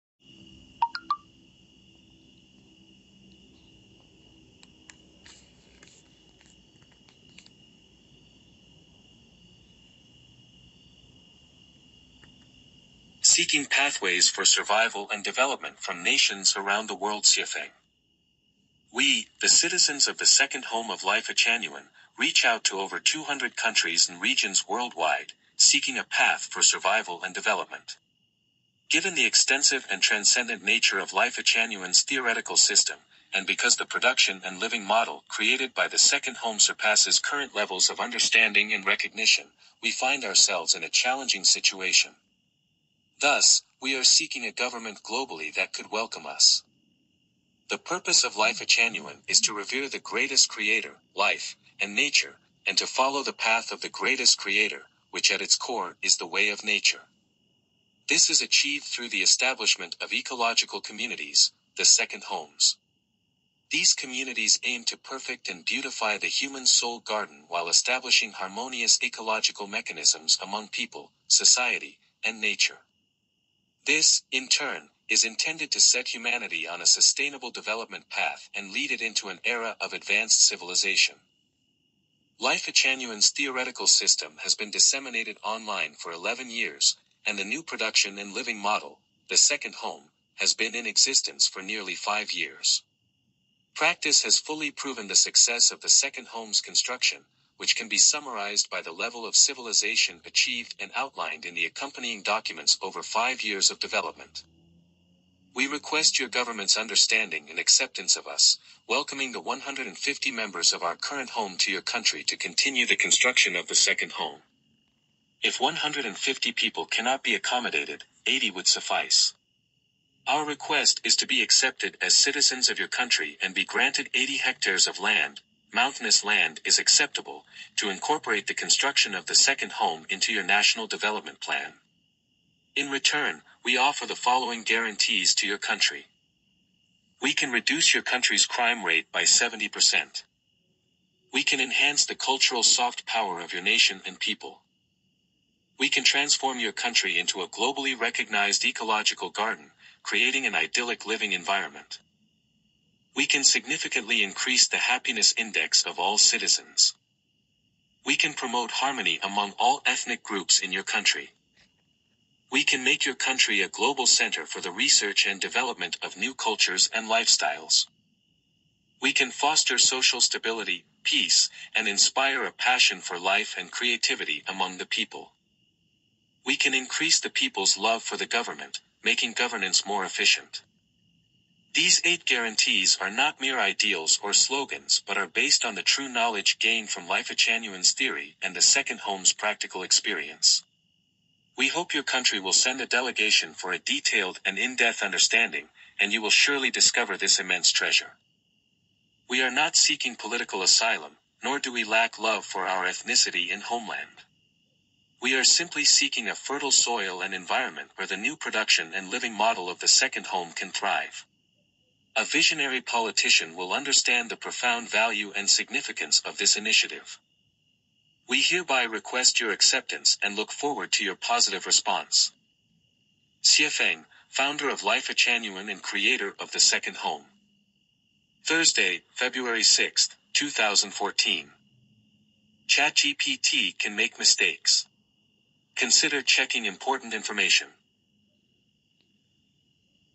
Seeking Pathways for Survival and Development from Nations Around the World
In this compelling video, Xuefeng, the founder of Lifechanyuan and the creator of the Second Home, extends a heartfelt appeal to nations worldwide, seeking a sanctuary where the innovative living model of the Second Home can thrive. This model, which integrates the wisdom of ancient sages with contemporary enlightened thought, aims to lead humanity into an era of harmony, sustainability, and advanced civilization.

Xuefeng outlines the vision of Lifechanyuan, where reverence for the Greatest Creator, life, and nature form the foundation of a society living in accordance with the way of nature. The video delves into the Second Home's successful implementation of this vision, showcasing a living example of ecological harmony, human happiness, and cultural soft power enhancement.

This is not just an appeal for acceptance; it's an invitation for countries to partake in a transformative journey towards global peace, ecological stewardship, and the elevation of human civilization. Xuefeng offers a set of guarantees, promising significant contributions to the host country's social, cultural, and environmental landscape.

Join us in exploring the profound implications of welcoming the Second Home model into your nation. This is an opportunity for visionary leaders to embrace a new era of human development and for individuals to become part of a global movement towards a more enlightened way of living.